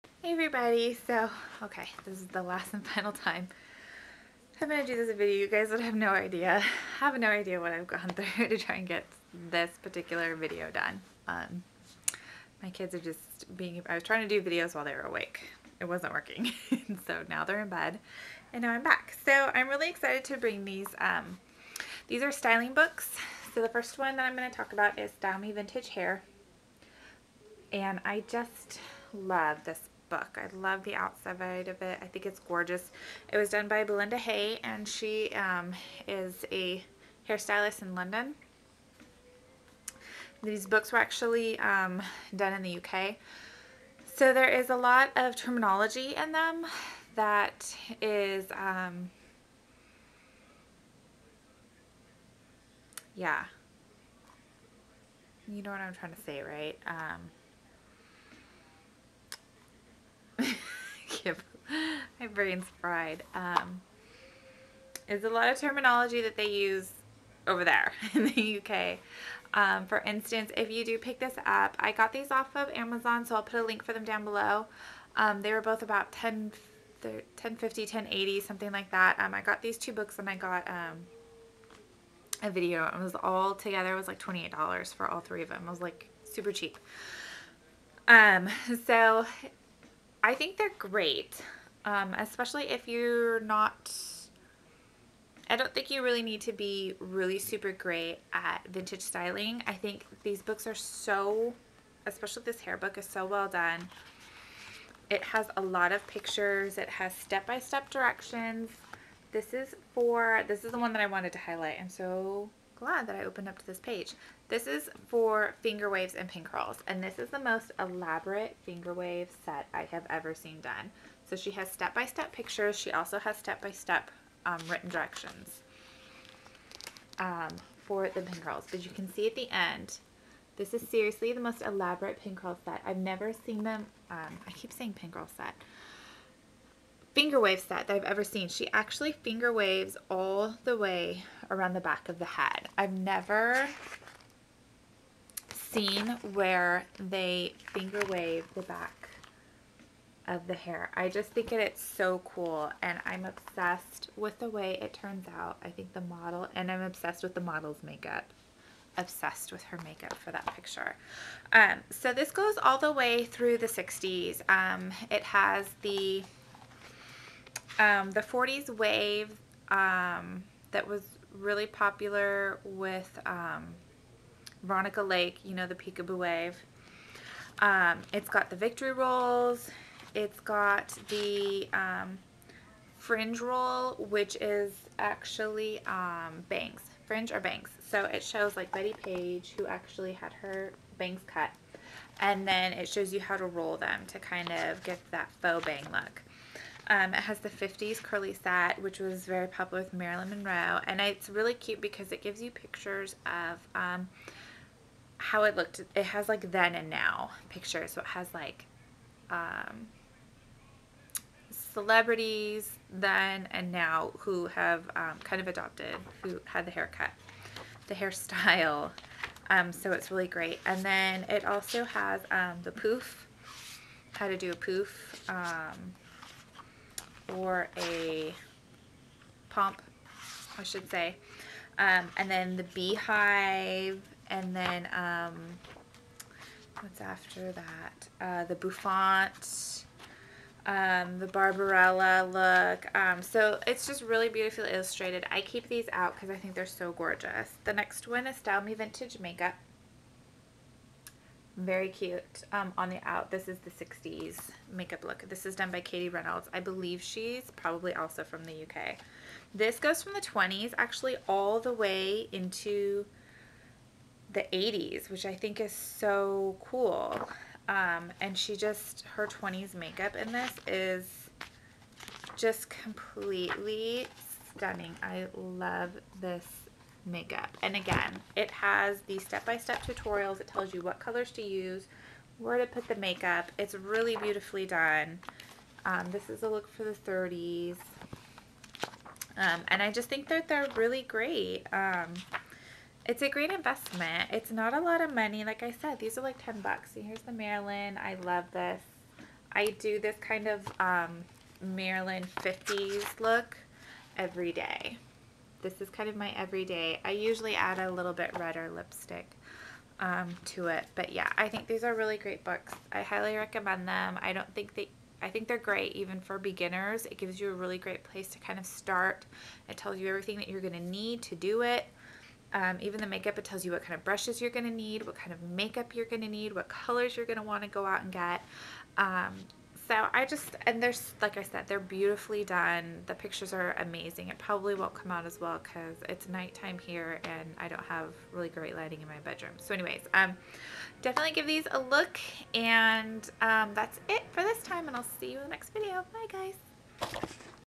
Style Me Vintage:Hair & Style Me Vintage:Makeup Books
Both were purchased from Amazon.com

Style Me Vintage:Hair

Style Me Vintage:Makeup

Both books are totally amazing! I really feel like even if your new to makeup and hair styling in general, you could still get a lot out of these books. Each style is broken down into several steps telling you exactly what to do and has tons of pictures to go with it. Plus they are super affordable books so you won't break the bank investing in them!